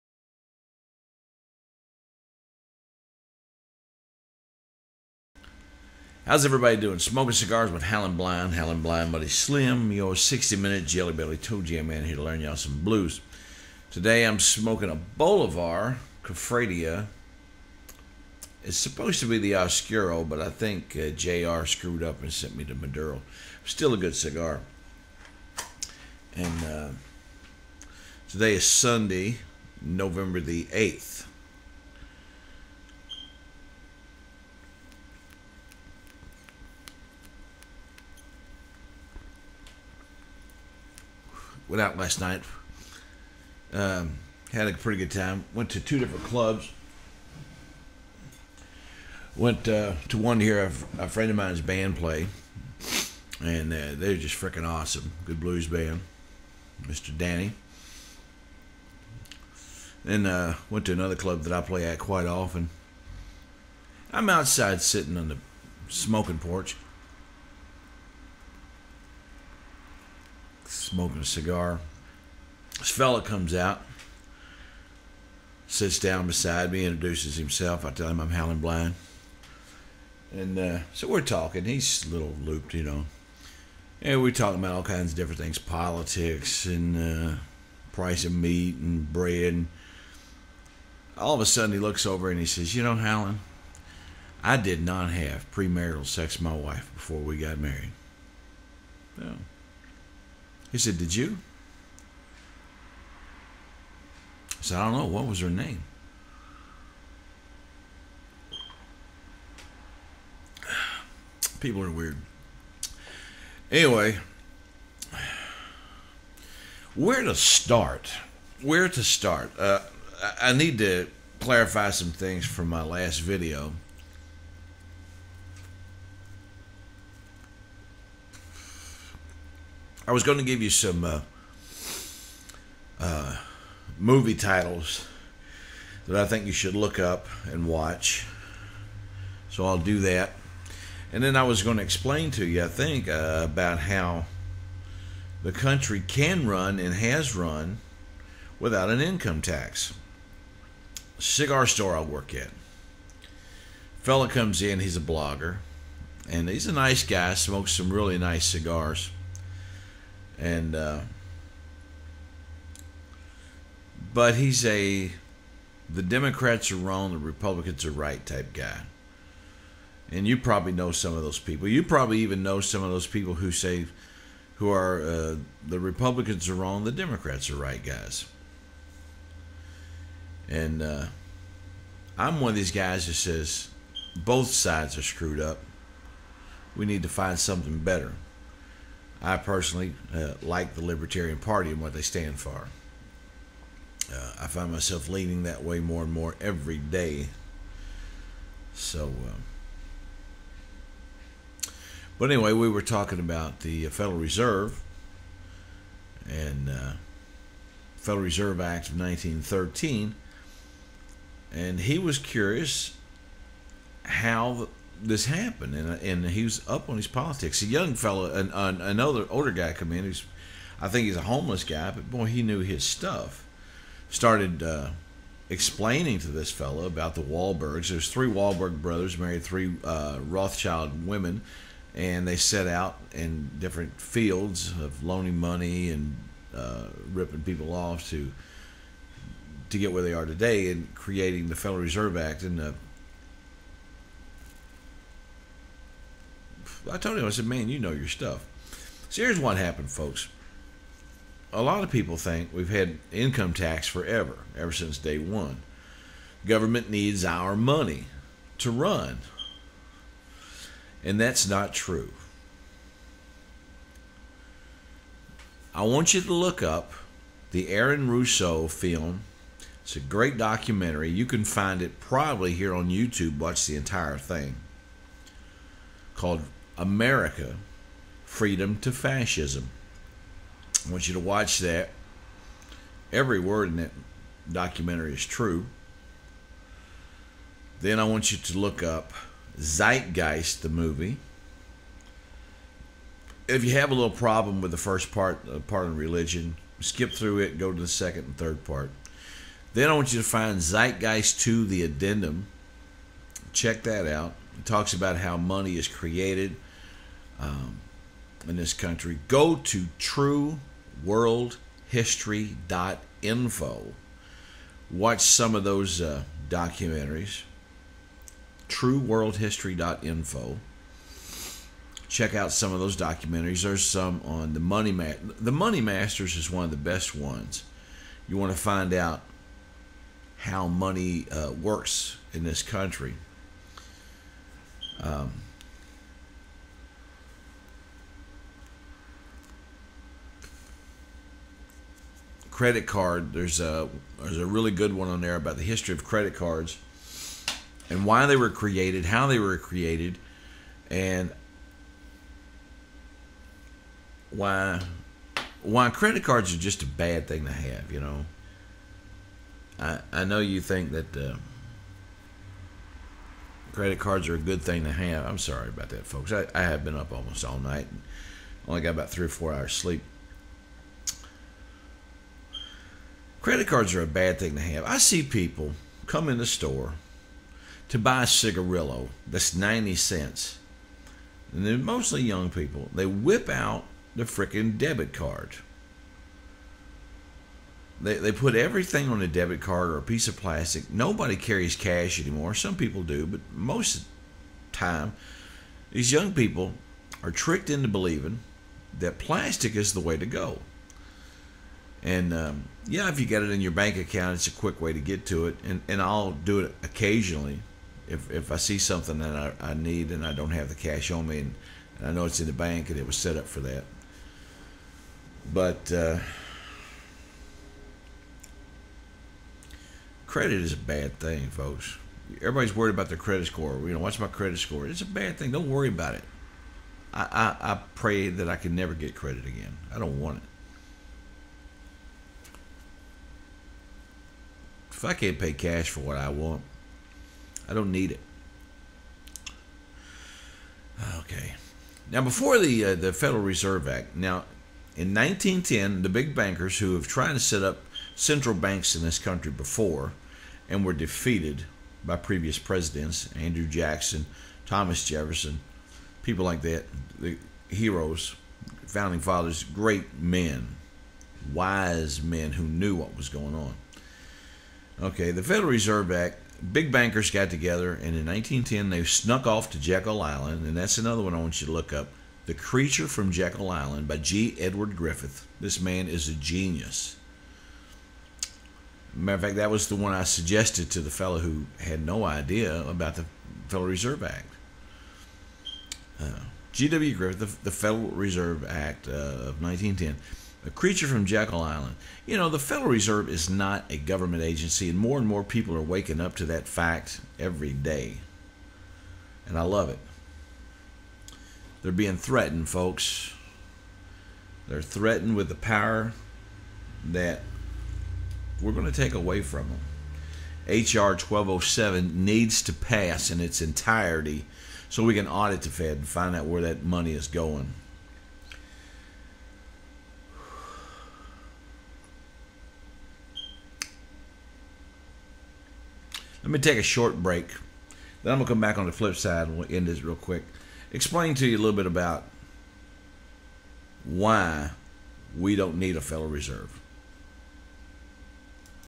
Smoking Cigars with Howlinblind:The Fed and some movies part 1
Some great movies to watch and Why we dont need the federal reserve